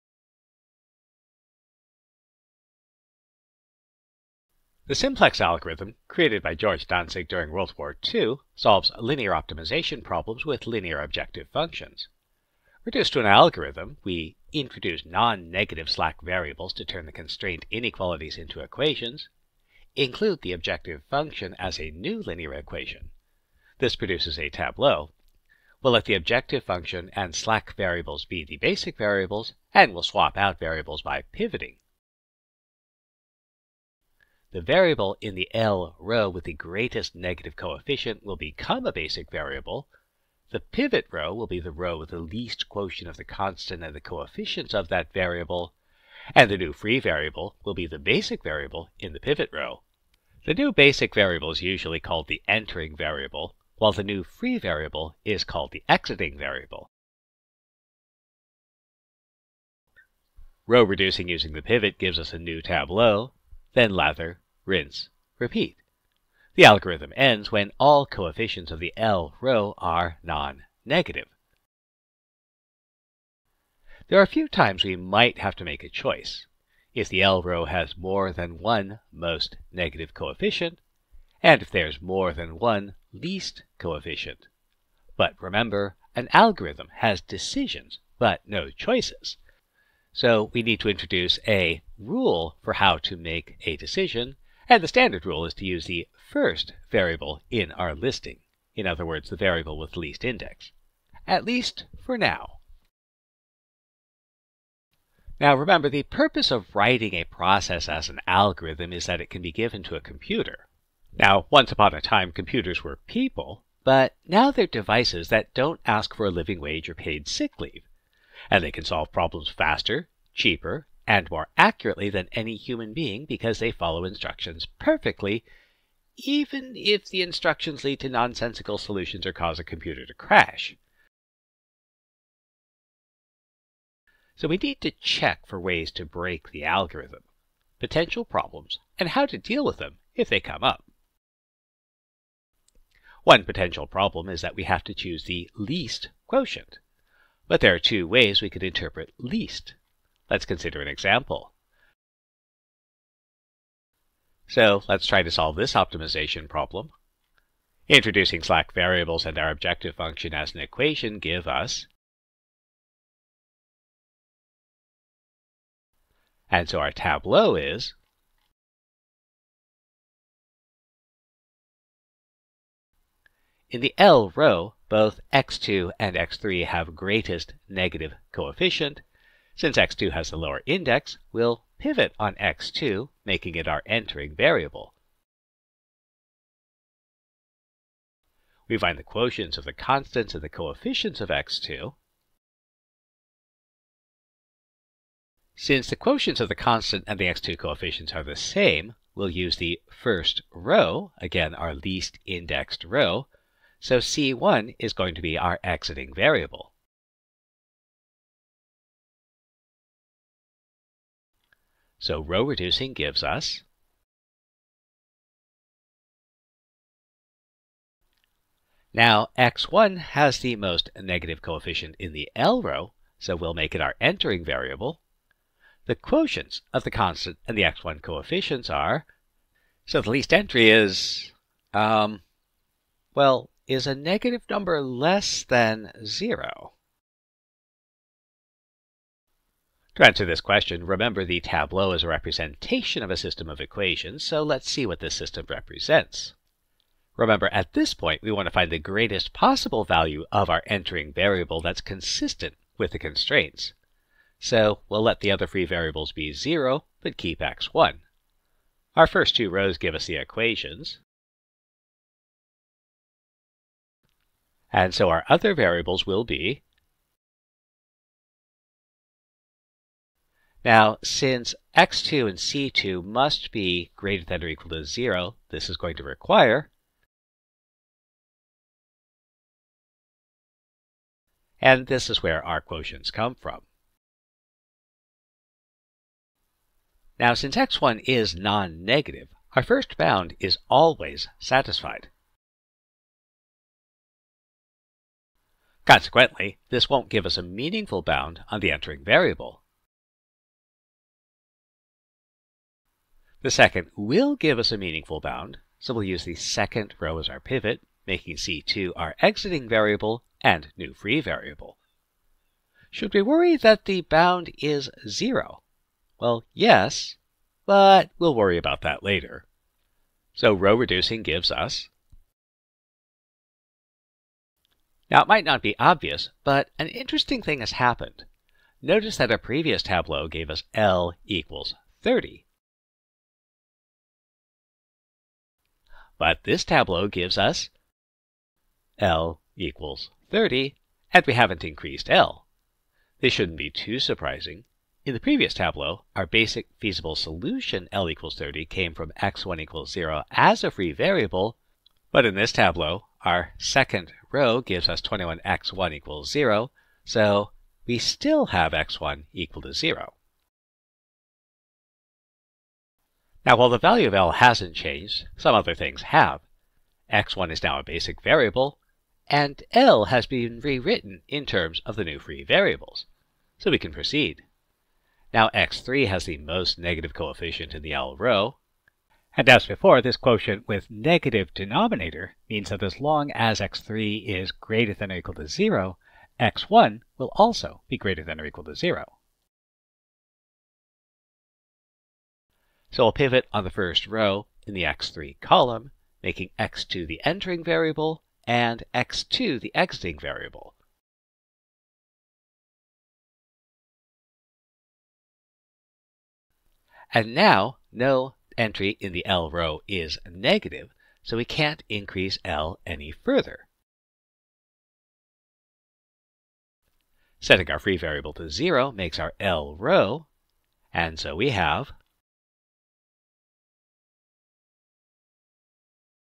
The Simplex Algorithm
Solving linear optimization problems using the simplex algorithm.